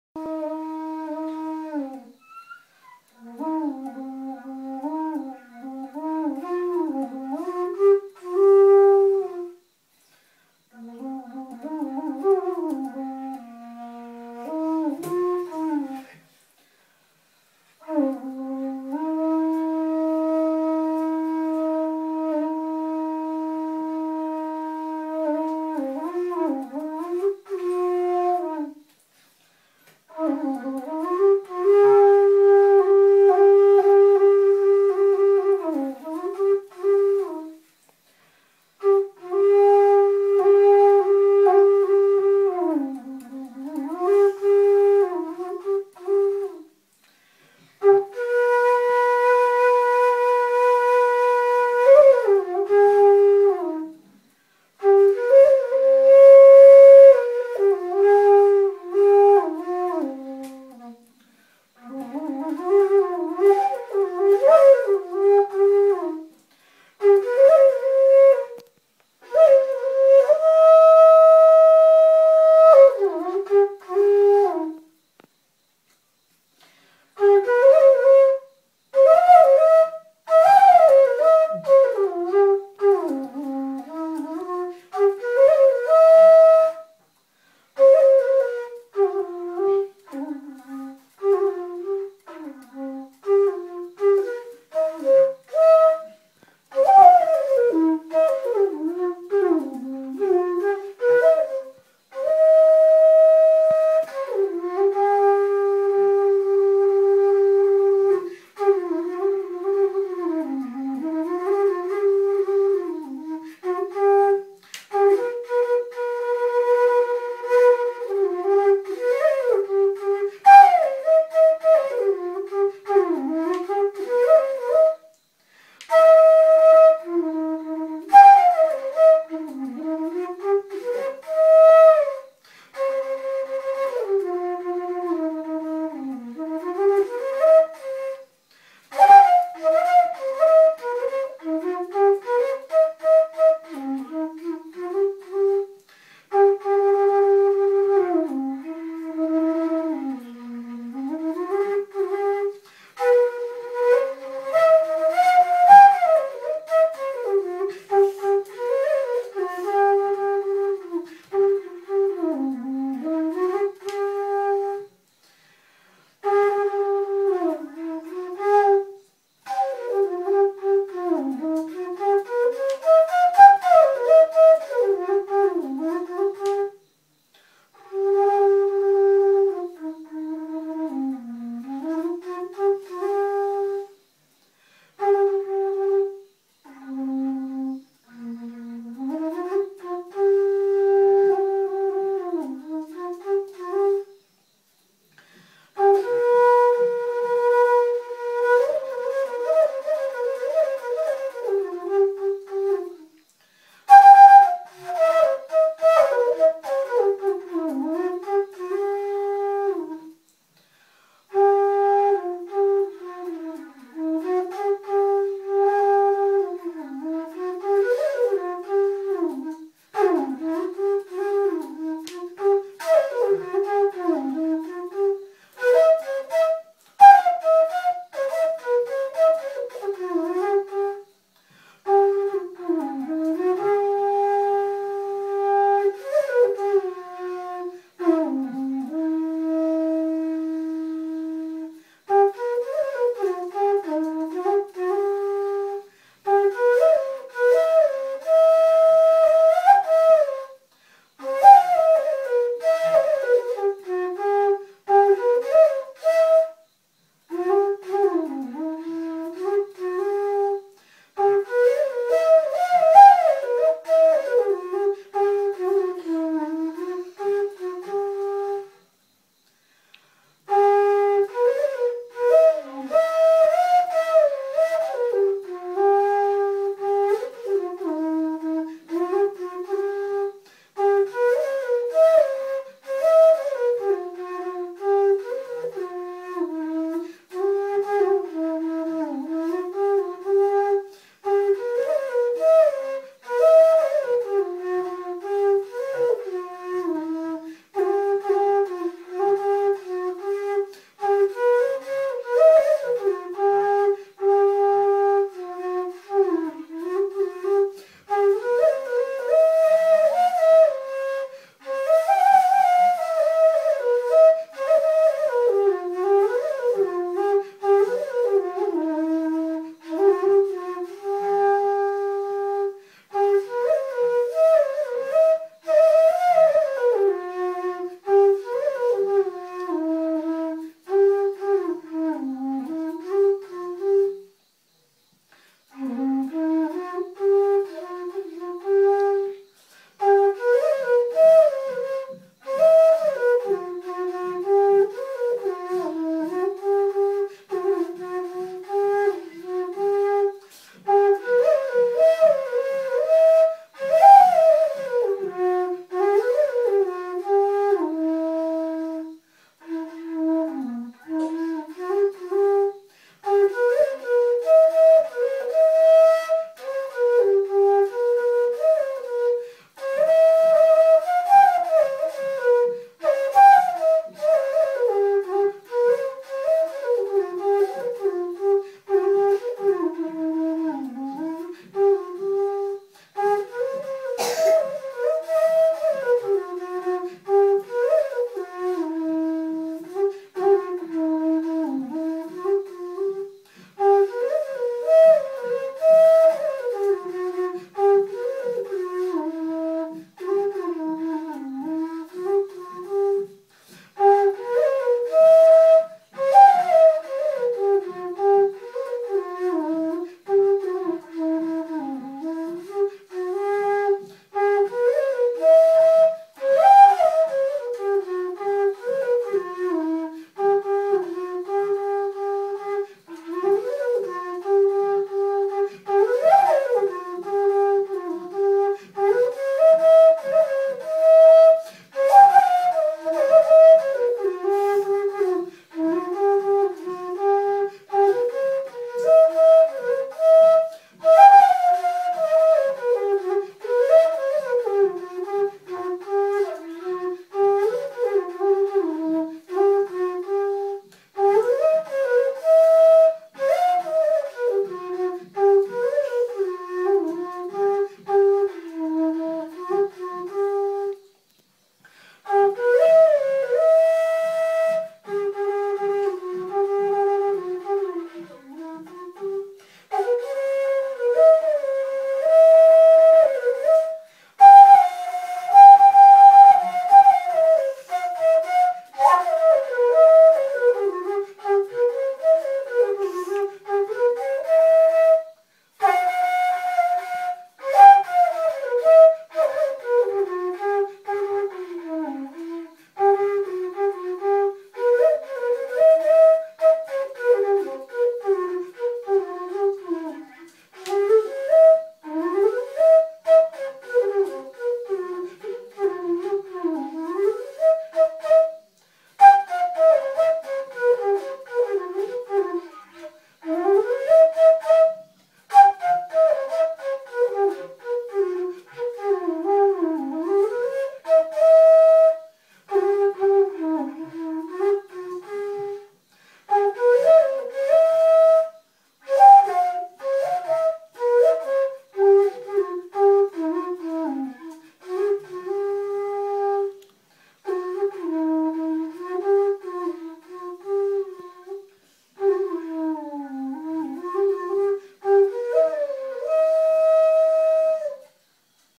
SURESH GORE.- FLUTE
Mr. Suresh Gore who liven in a small village called Piliyooru (Phandije) in Mangalore dist. He is a multi- talented person who loves music and artistic work. His occupation is silver art and agriculture, where as music is his passion.'
He not only plays flute but also prepares different kinds of flutes including Bansuri which is a rare talent.